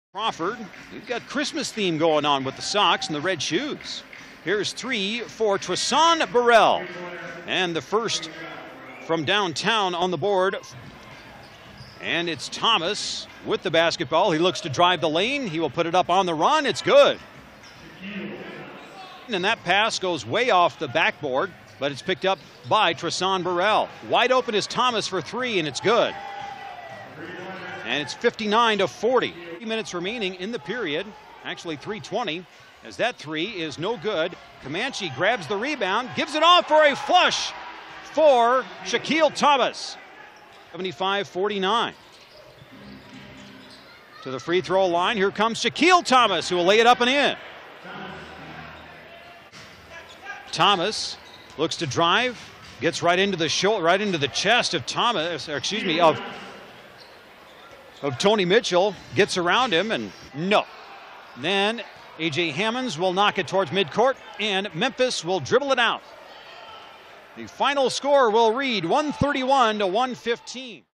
MHU@SXF: Thomas adds 11pts
Shaquille Thomas posts 11points on 5-of-8 shooting from the field against the Skyforce.